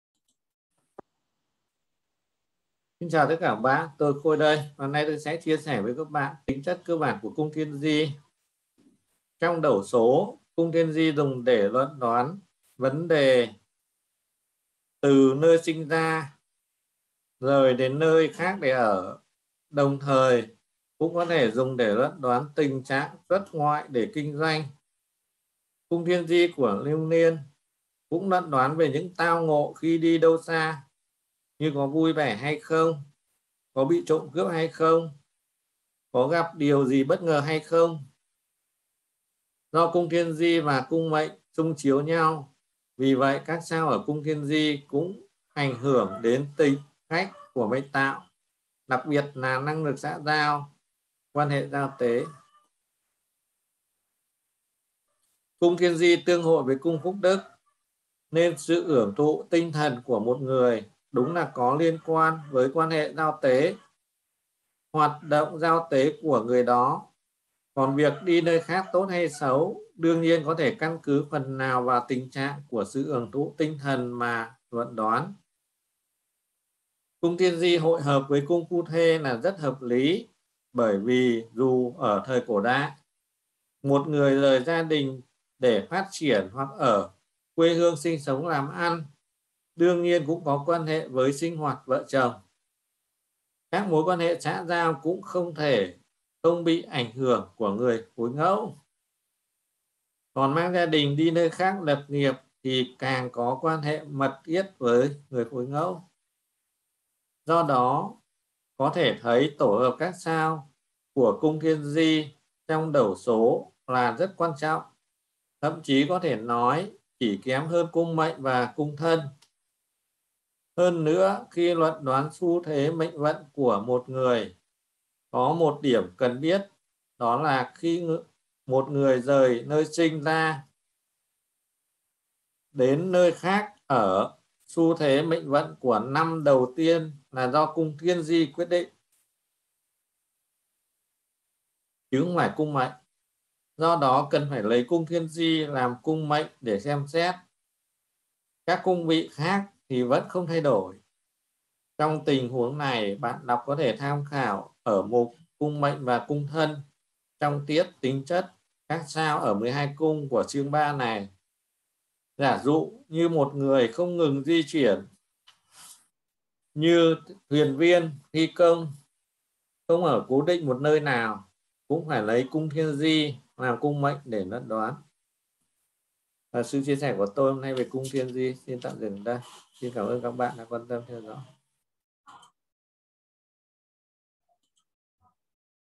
Cung Thiên Di
Ý nghĩa cơ bản Cung Thiên Di. Nguồn ""Trung Châu Tử Vi Đẩu Số Tam Hợp Phái"" tập 1 tác giả Nguyễn Anh Vũ.
+ Đặt Mua Bộ sách Trung Châu Tử Vi Đẩu Số Tam Hợp Phái (Bộ 2 Cuốn)